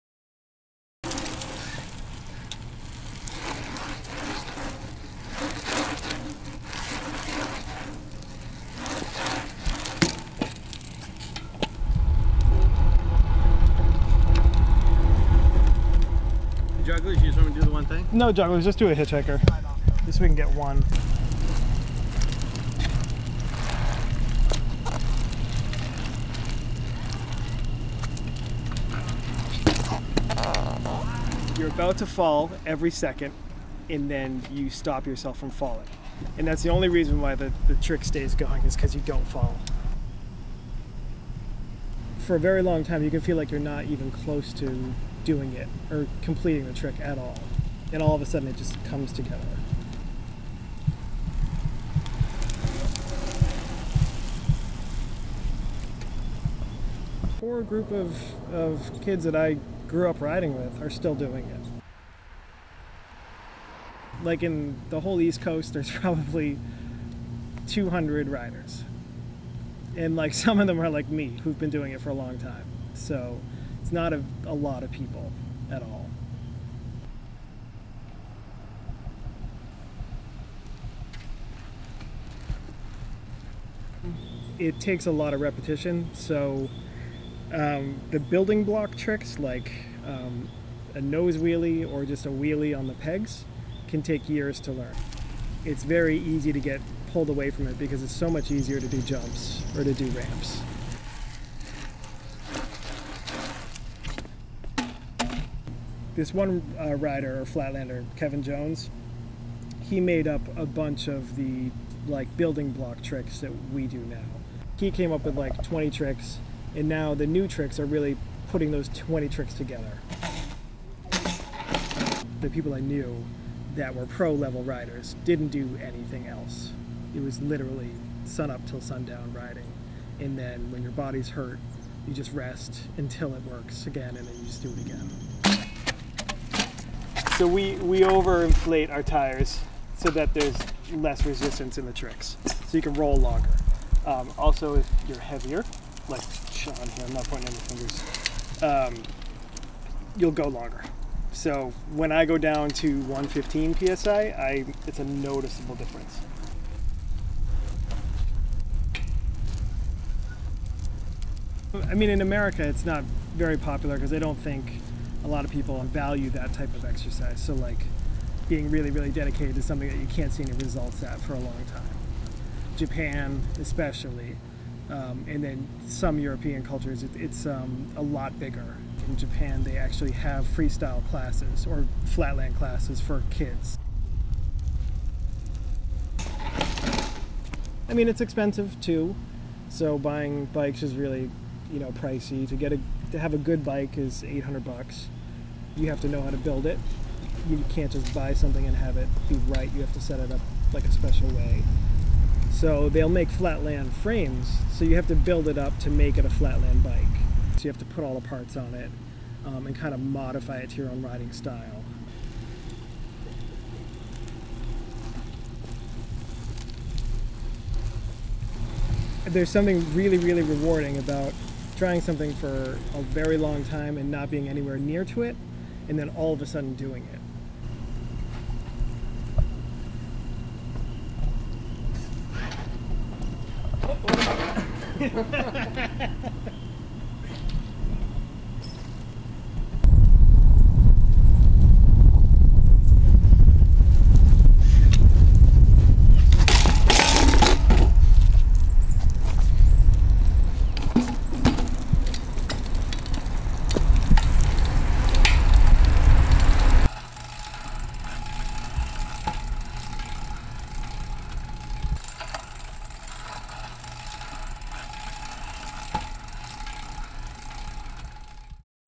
Flatland
A documentary short on flatland bmx riding.